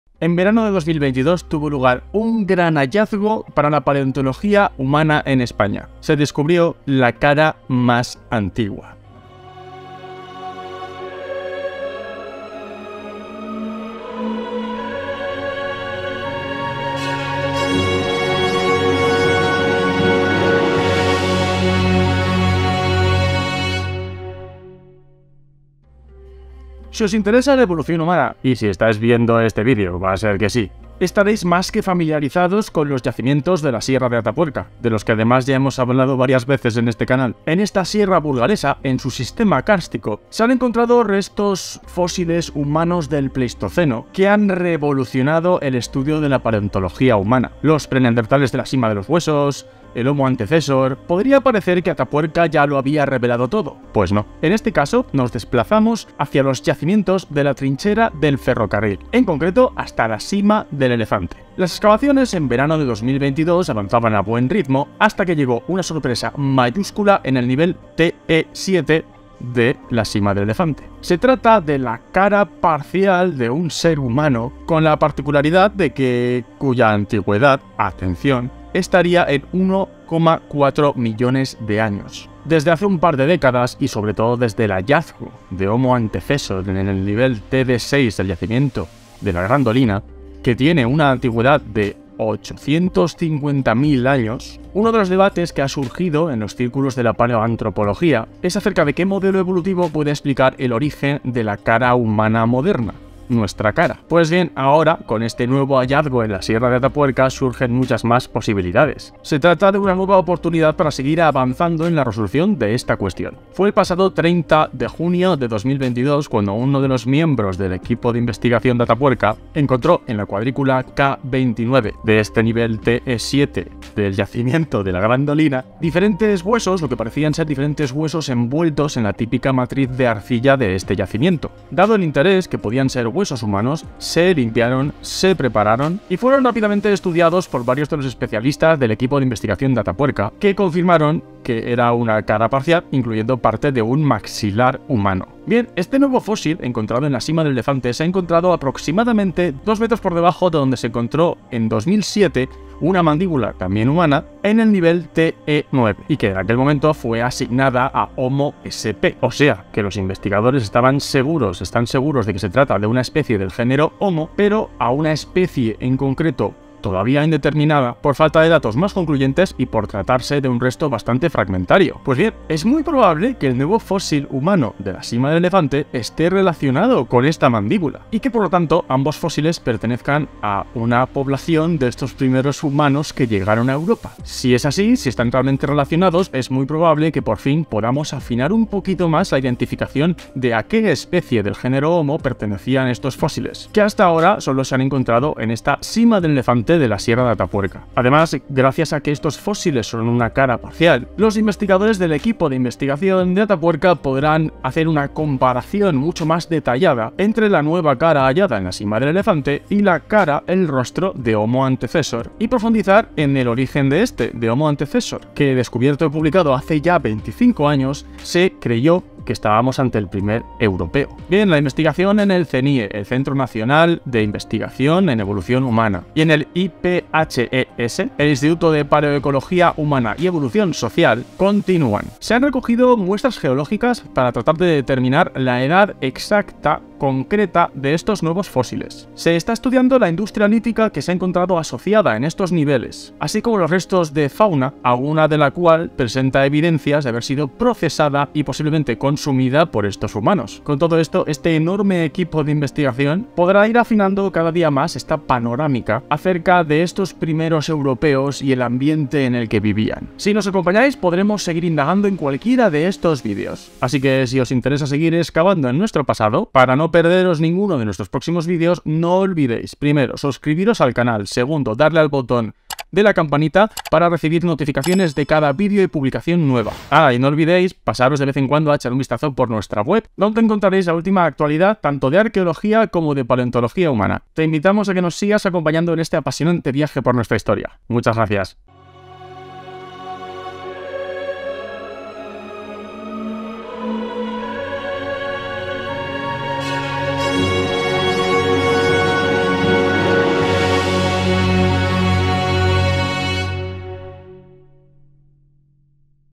La cara humana más antigua: el hallazgo en la Sima del Elefante (Atapuerca)
La paleontología humana ha dado un gran salto gracias a un descubrimiento histórico en España. En el verano de 2022, en el yacimiento de la trinchera del ferrocarril en Atapuerca, se ha encontrado la cara humana más antigua de nuestro país. Con una antigüedad de aproximadamente 1,4 millones de años, la cara del humano más antiguo de Europa, este hallazgo puede aportar nuevos datos sobre el origen de la cara humana moderna y sobre las primeras poblaciones humanas que habitaron Europa. El equipo de investigación del CENIEH y el IPHES continúa trabajando en este hallazgo para descubrir aún más sobre nuestra evolución humana. En este vídeo, te mostramos detalles sobre este emocionante descubrimiento y lo que significa para la comunidad científica. No te lo pierdas.